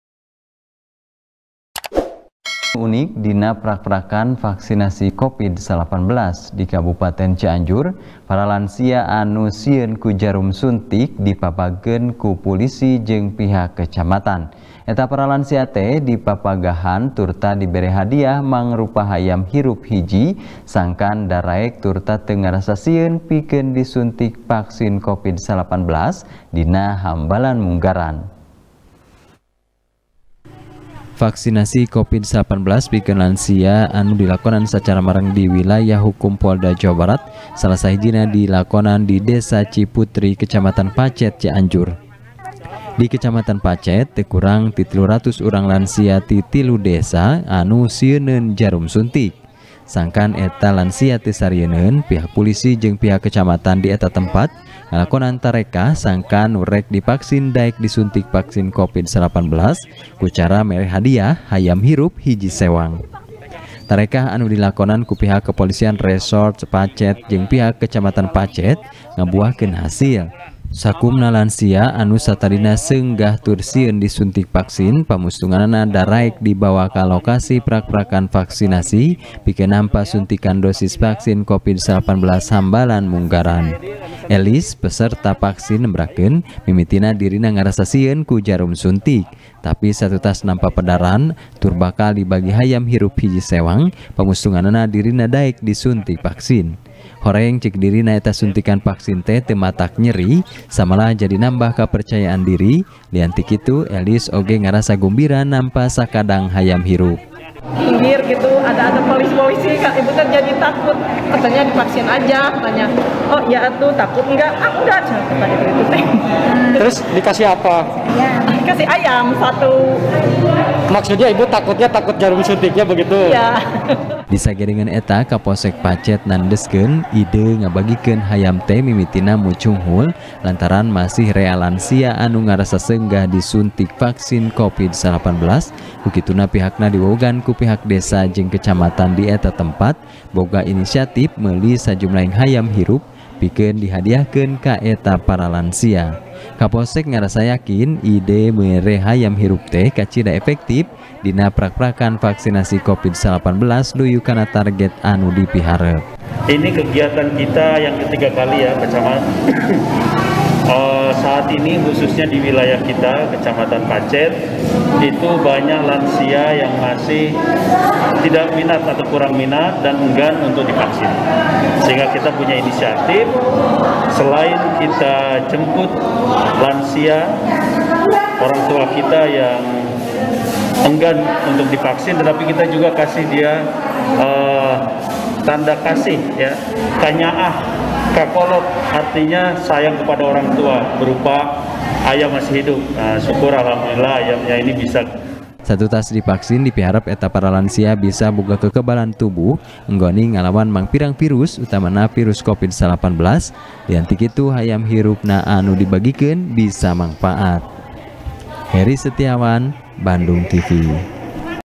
Unik, Satutas Divaksin, 300 Urang Lansia Nampa Hadiah Hayam Hirup | TP | BANDUNGTV
TP | BANDUNGTV | IWAN M RIDWAN | Aya Nu Unik Dina Prakrpakan Vaksinasi Covid-19. Di Kabupaten Cianjur. Para Lansia Anu Sieun Ku Jarum Suntik, Dipapagkeun Ku Pulisi Jeung Pihak Kecamatan. Eta Para Lansia Teh Dipapagahan Turta Dibere Hadiah Mangrupa Hayam Hirup Hiji, Sangkan Daraek Tur Teu Ngarasa Sieun Pikeun Disunti Vaksin Covid 19 Dina Hambalan Munggaran.